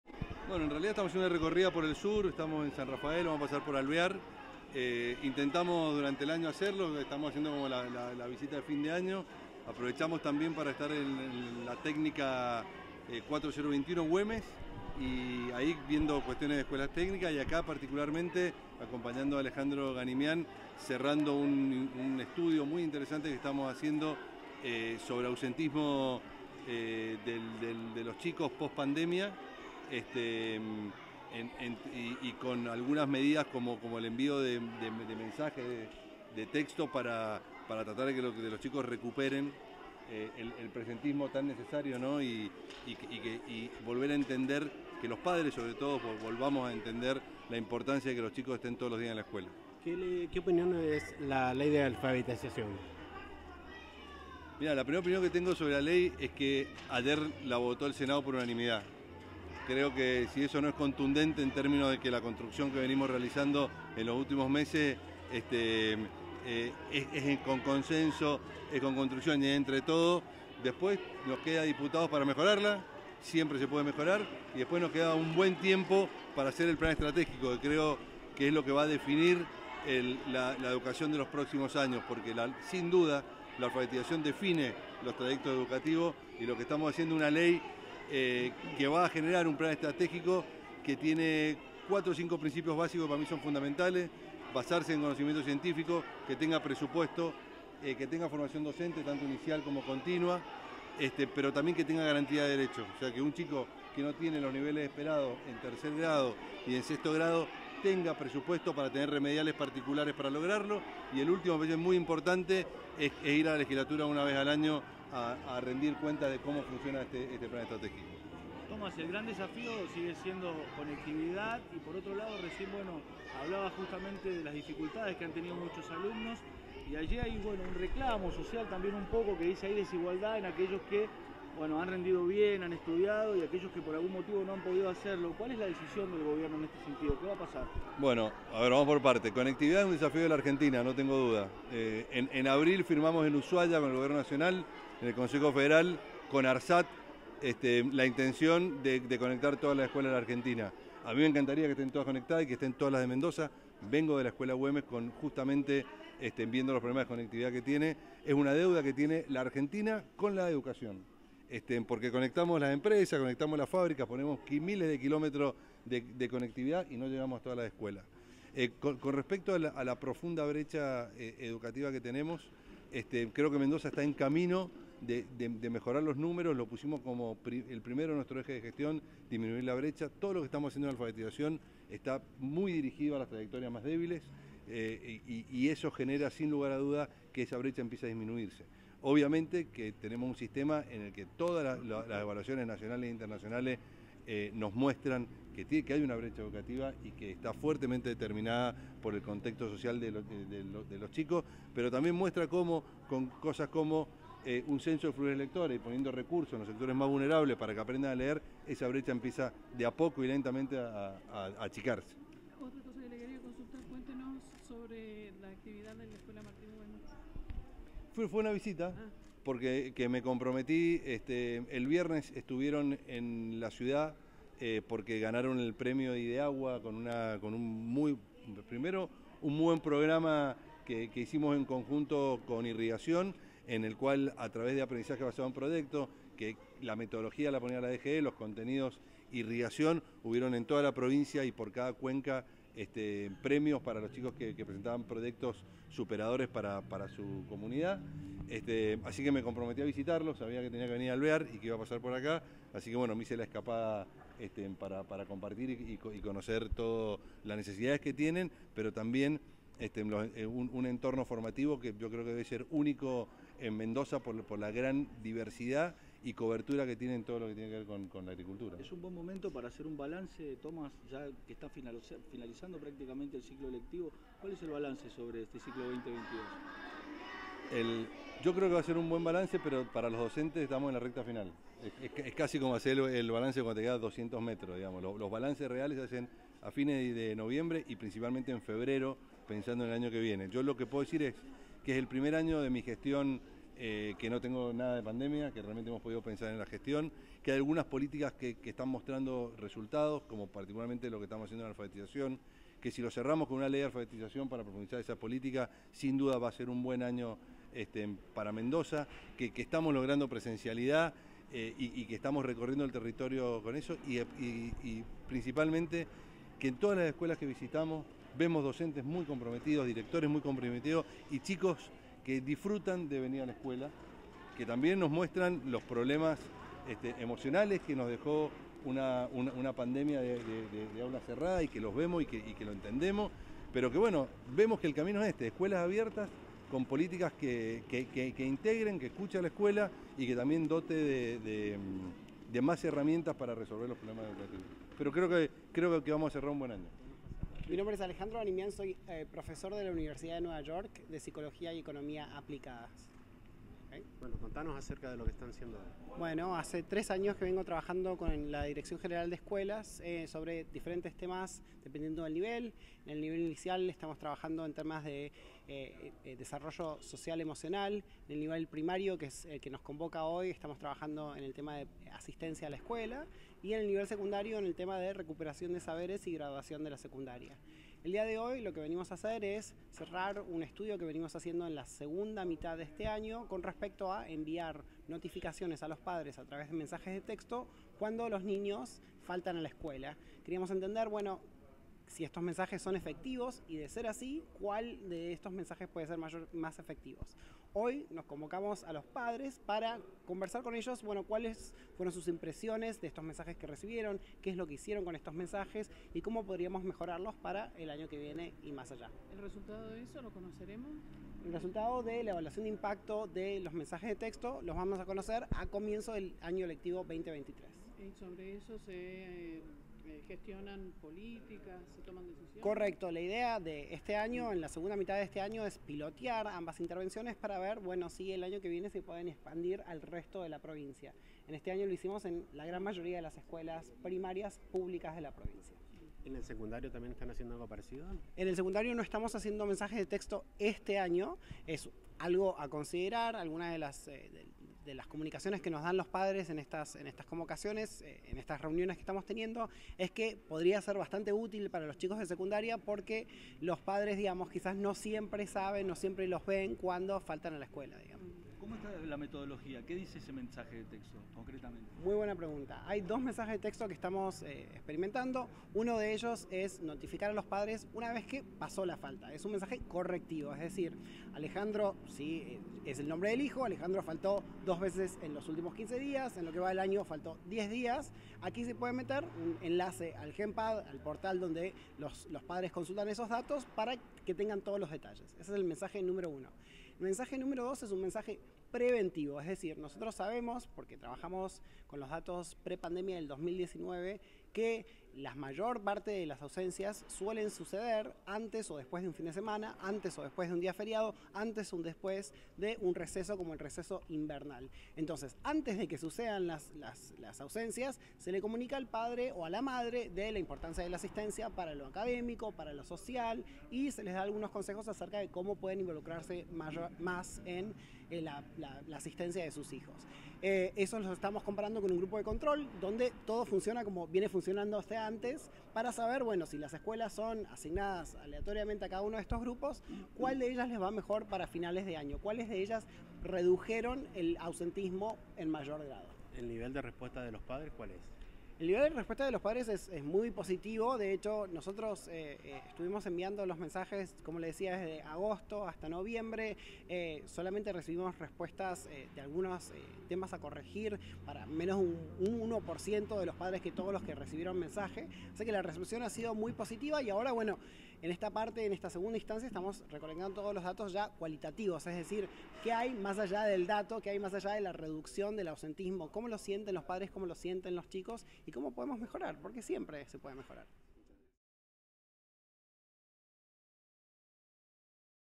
Thomas recorrió escuelas en San Rafael y Alvear
El titular de la DGE dialogó con diferentes comunidades educativas del Sur mendocino para compartir el trabajo cotidiano que realizan escuelas, familias y estudiantes.